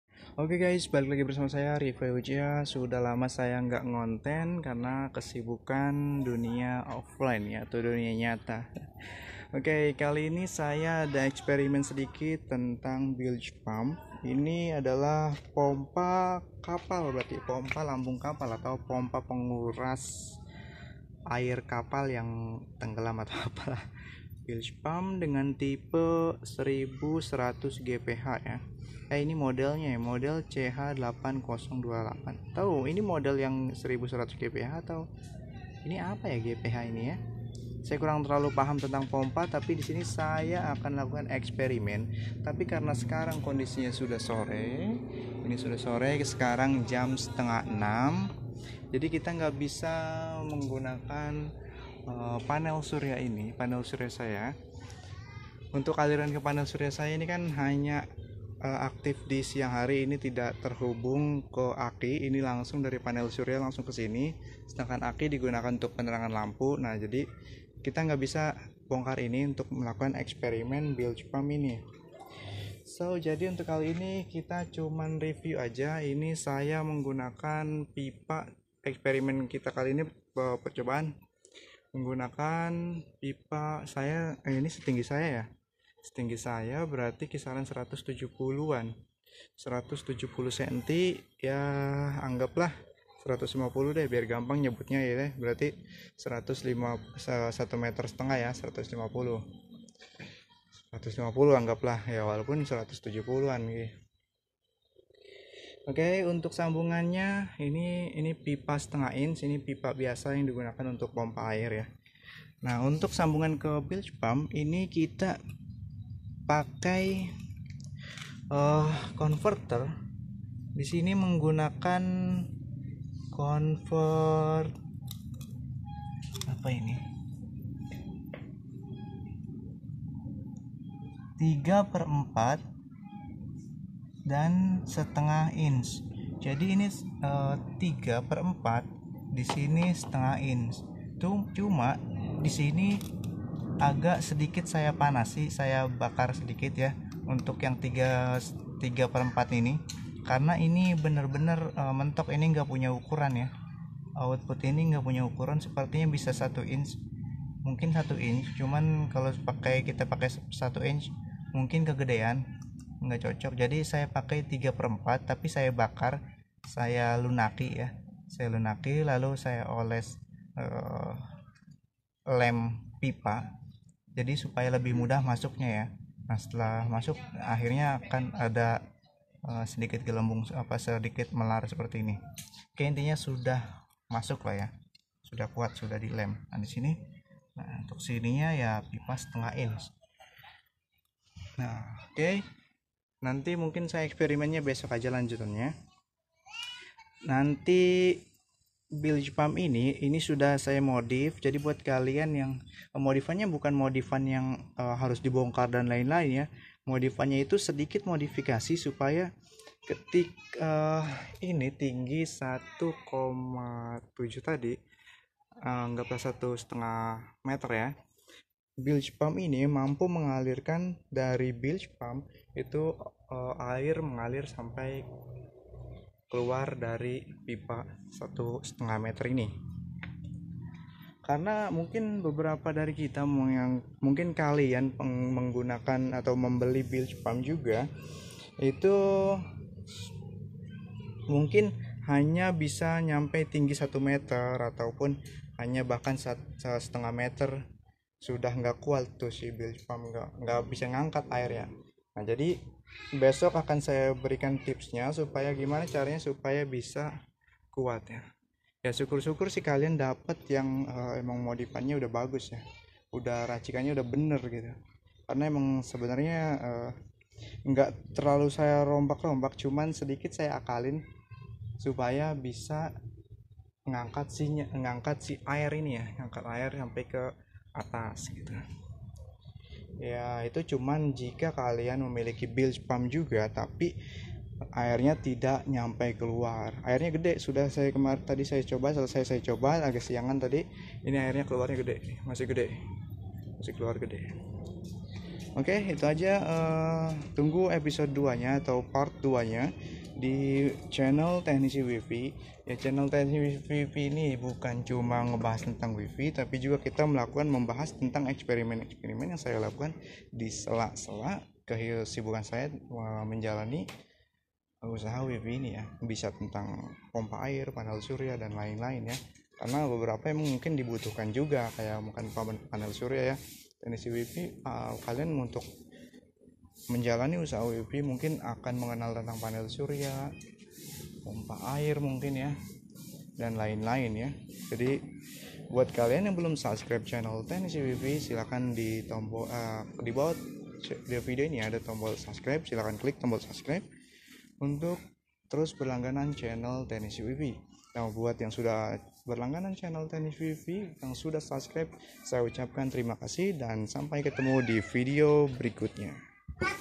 cara merakit bilge pump 1100GBH untuk pompa celup air sumur dangkal
Baru selesai rakit pompa air menggunakan bilge pump ternyata udah sore ga keburu di jajal ke panel surya, jadi lanjut besok ya..
Sebagai informasi bilge pump ini adalah pompa air untuk lambung kapal yang kemasukan air, jadi sudah di desain untuk dapat mengalirkan air dengan debit yang sangat besar tapi memiliki tekanan yang kecil.
Jadi selain di gunakan untuk kapal/perahu sangat cocok juga untuk:
1. pengairan tanaman.
2. Menguras bak mandi.
3. Kuras empang.
4. Pompa sumur dangkal.
5. Dan keperluan lainnya yang memiliki ketinggian tidak seberapa dari permukaan air.